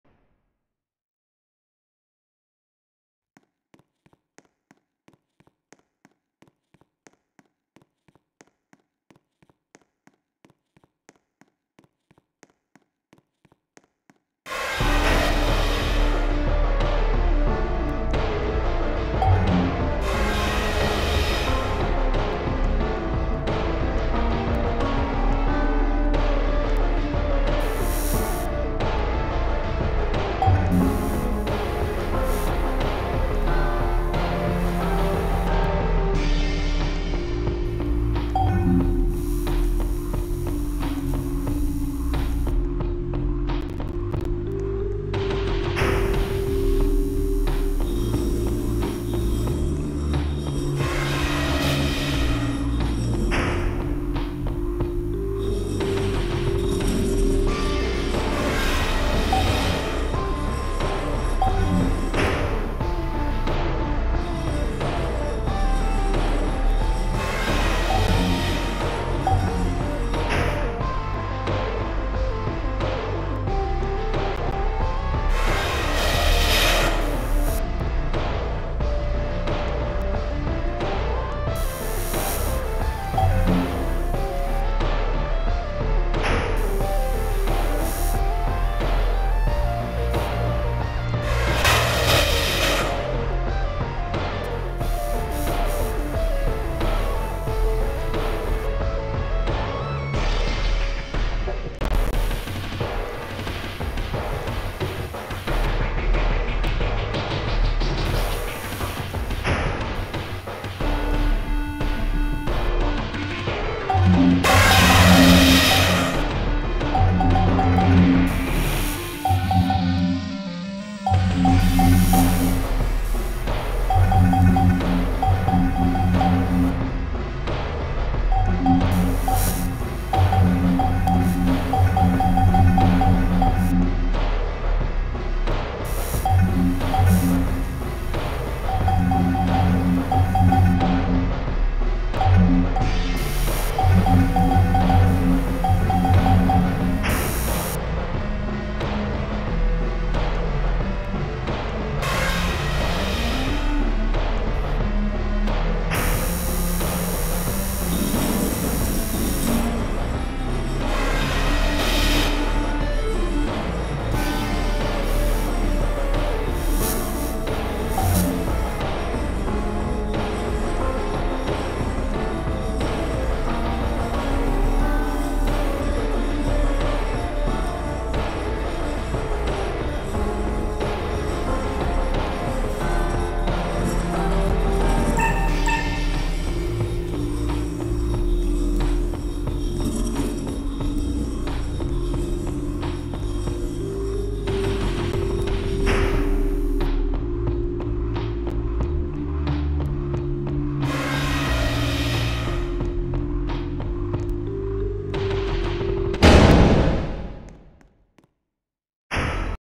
6:06 PM: Darker than the maze (buffed ftw lap 2)
hi so, i also did this itemless just for your own amusement, and also since you guys like to see me torture myself. like, i dont get anything for doing this. you guys want me to bring unspoken speed up next? im also gonna call the entity known as the raging hunter